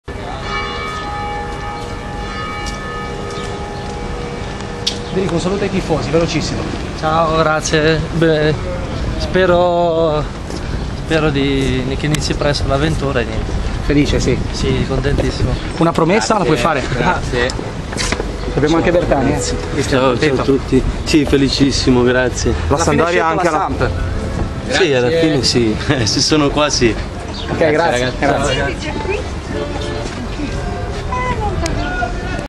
Piovaccari e Bertani, visite mediche al Baluardo.MP4
Primo giorno in blucerchiato per  Piovaccari-Bertani, la nuova coppia-gol della Samp (Gianluca Tropella-Sportmedia)